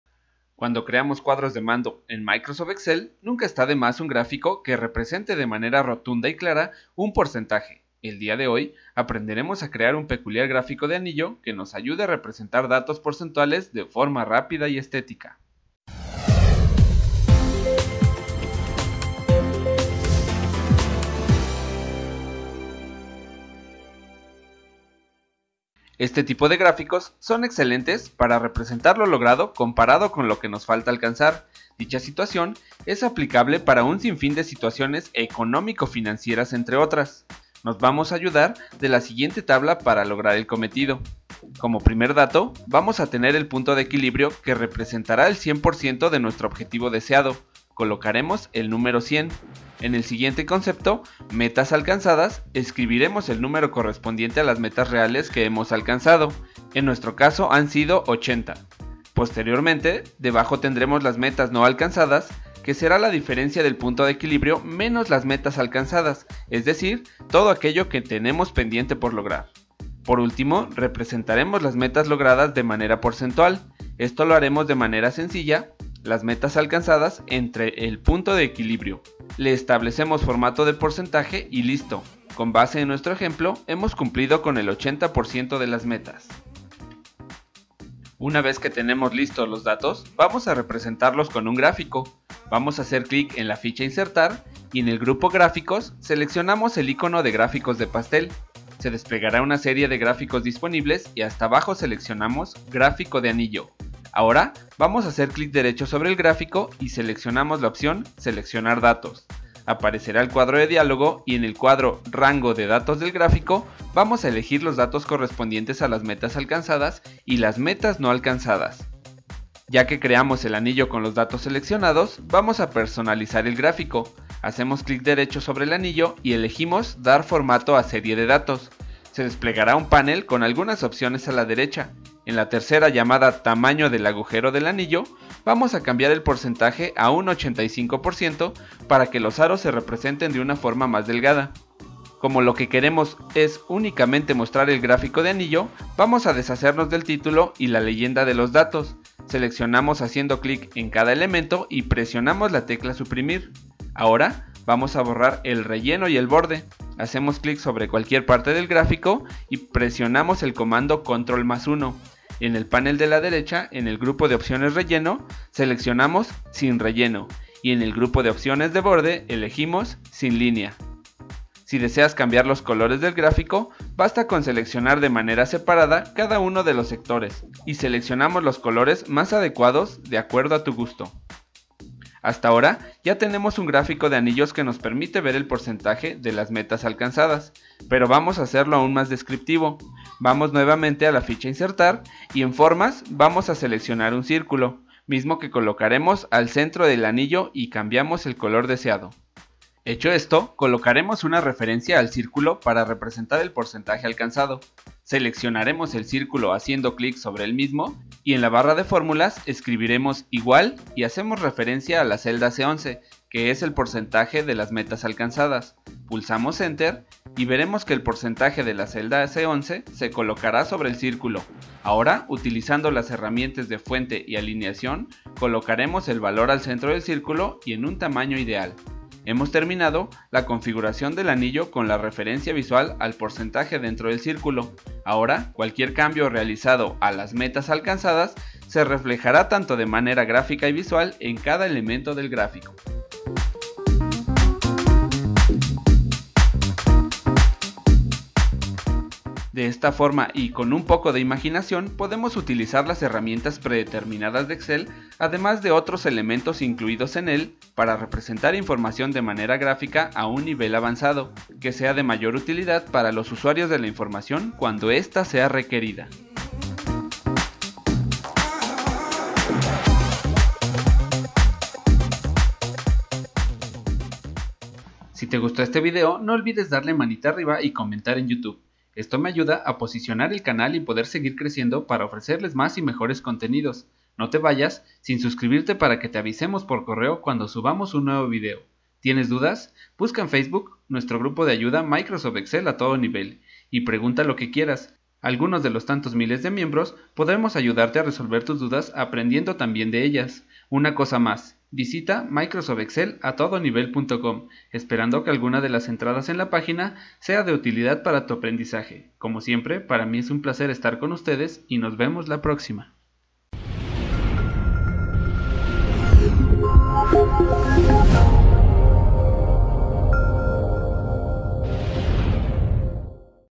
GRÁFICO DE ANILLOS CON PORCENTAJE EN EXCEL | Tips y trucos en Excel
Tu nivel de Excel a Nivel dios 🚀 en nuestra academia de Excel y Finanzas 💹🎓

En este video aprenderemos a crear un grafico de anillos con la ayuda visual de un circulo que mostrará el porcentaje de manera dentro del gráfico.